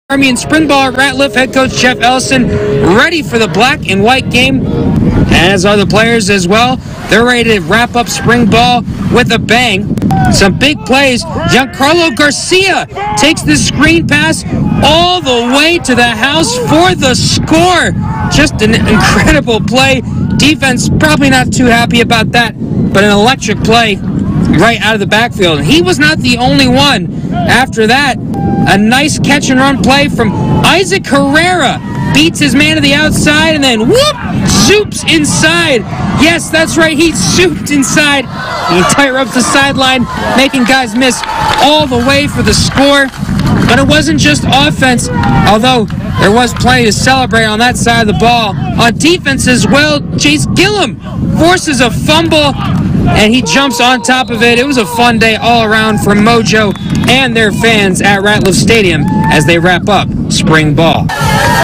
Permian spring game
Permian wraps up spring ball with their Black vs White game.